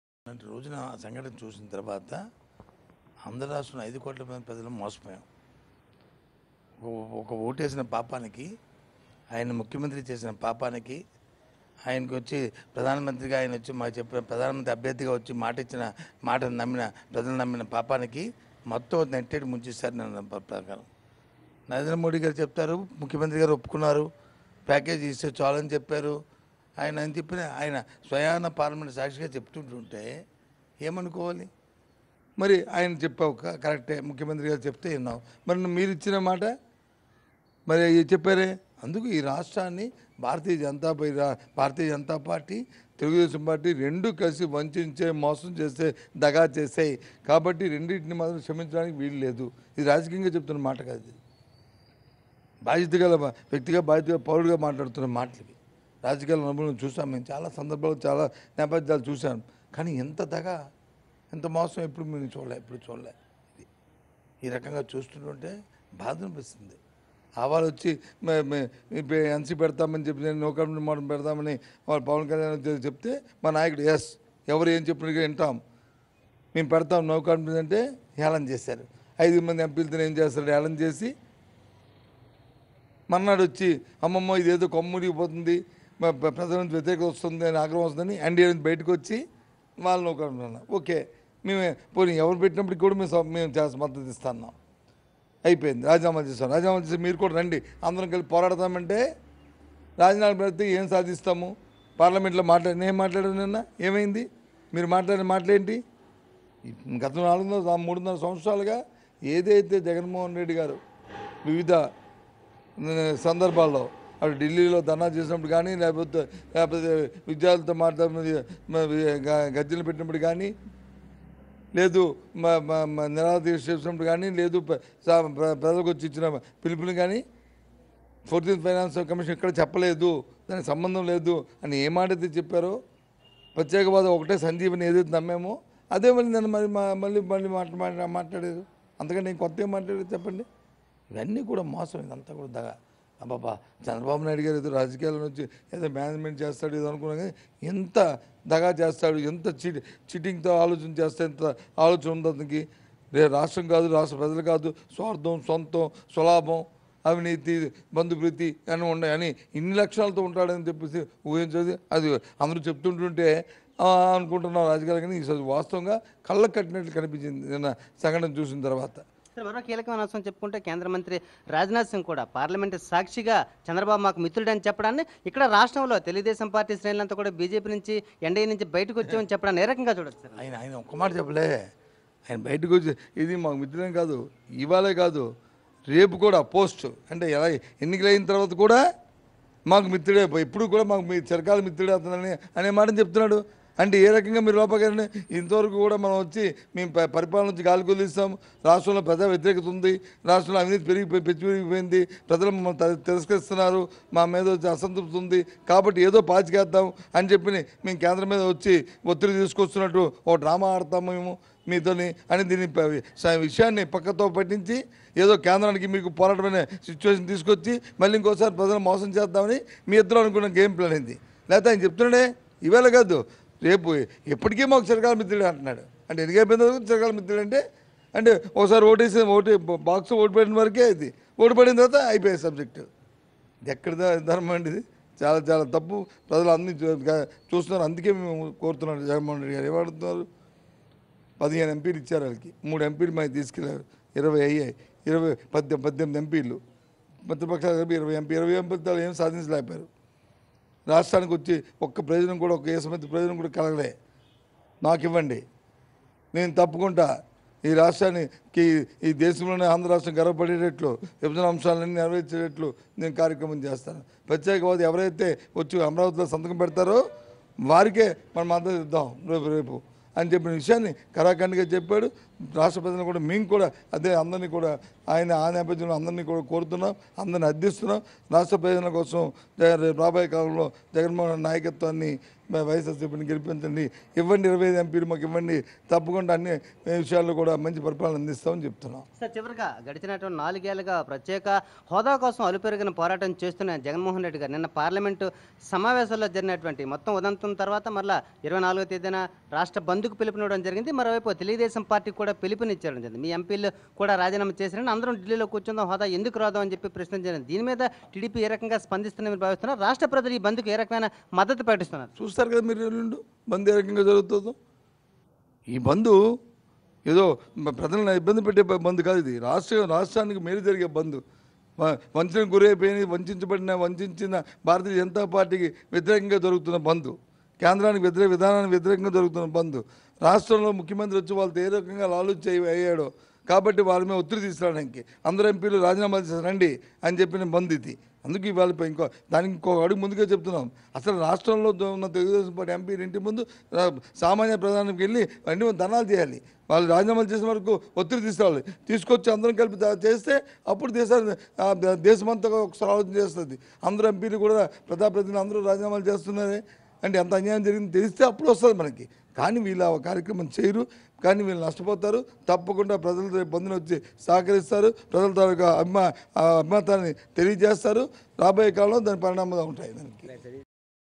YSRCP Senior Leader Botsa Satyanarayana comments on TDP Drama in Parliament
YSRCP Senior Leader Botsa Satyanarayana comments on TDP Drama in Parliament - 21st july 2018